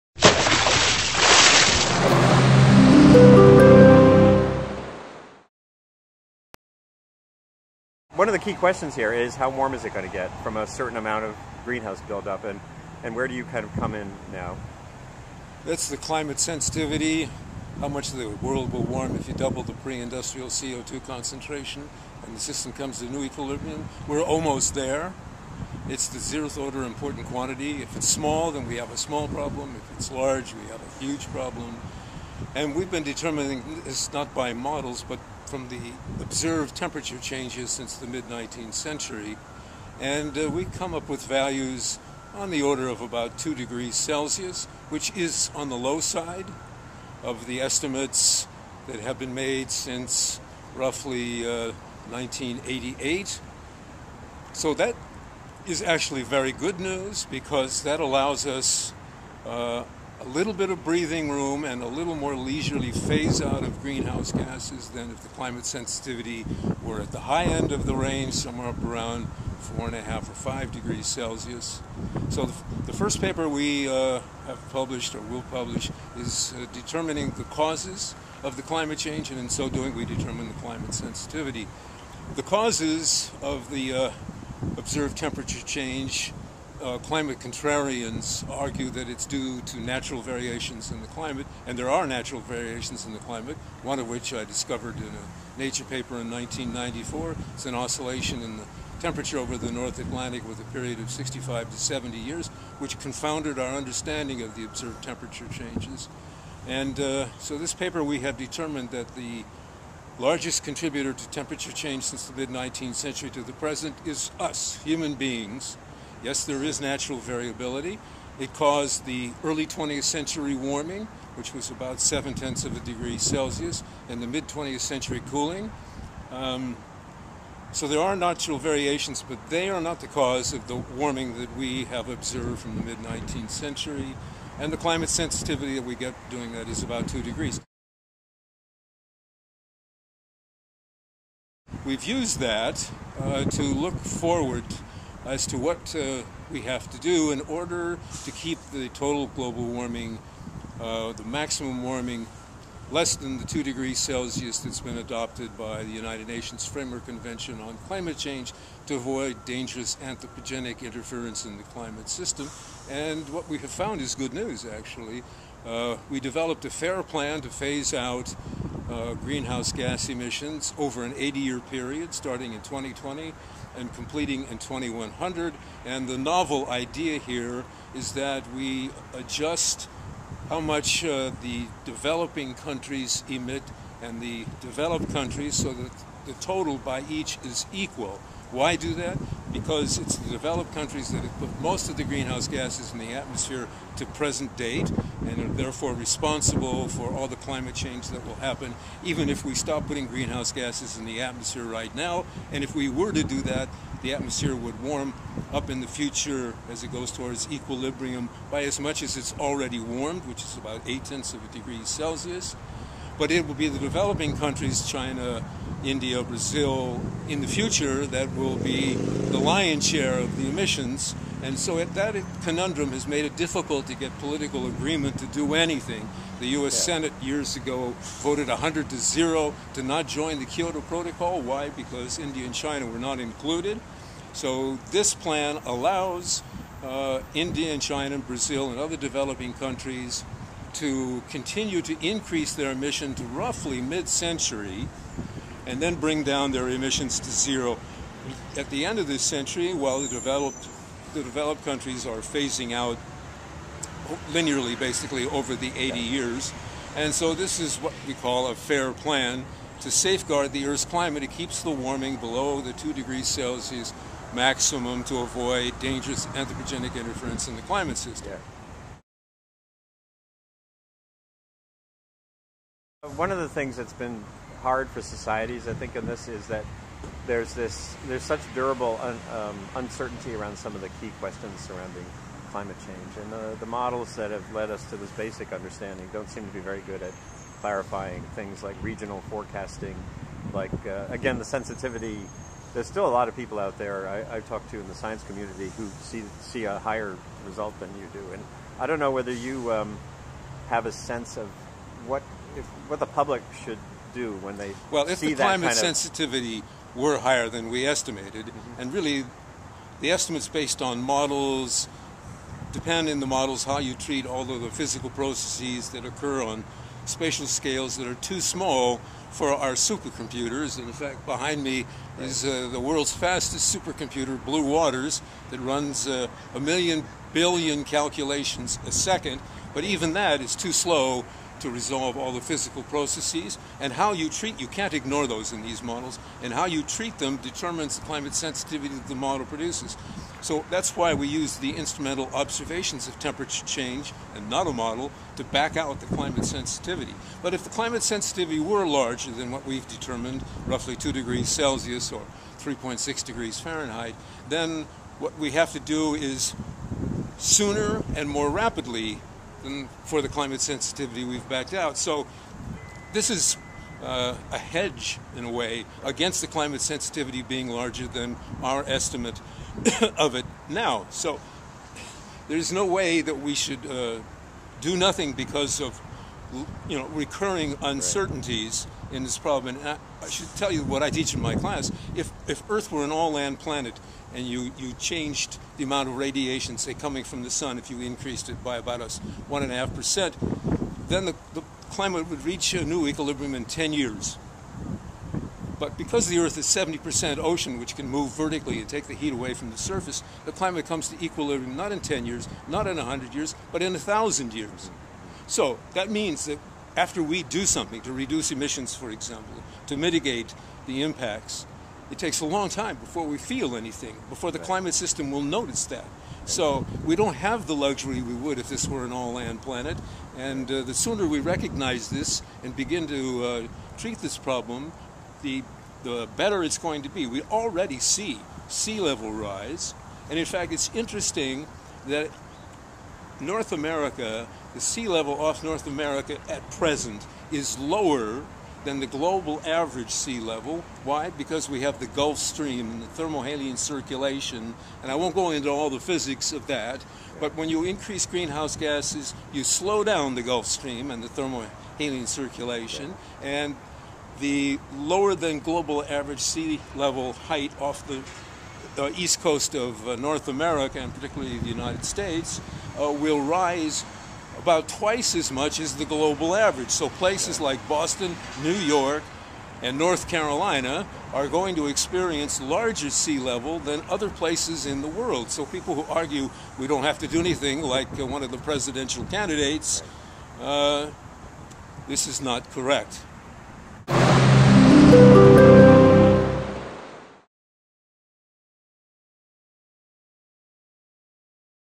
A Climate Scientist Sees a Fair Path for West and East on CO2
Michael Schlesinger, a climatologist at the University of Illinois, has been immersed in both climate science and policy analysis for decades. In this interview for Dot Earth, he summarizes findings in two recent papers on which he is a co-author (references below).

One paper concludes that the sensitivity of the climate system to the greenhouse-gas buildup could be lower than some other recent studies find.

The other charts a timetable by which the United States and other industrialized countries, followed by developing giants like China, can fairly apportion and time the task of decarbonizing their energy supplies.

"Causes of the Global Warming Observed since the 19th Century"
Michael J. Ring, Daniela Lindner, Emily F. Cross, Michael E. Schlesinger
Atmospheric and Climate Sciences
DOI: 10.4236/acs.2012.24035

"A Revised Fair Plan to Safeguard Earth's Climate"
Michael E. Schlesinger, Michael J. Ring, Emily F. Cross
Journal of Environmental Protection
DOI: 10.4236/jep.2012.310151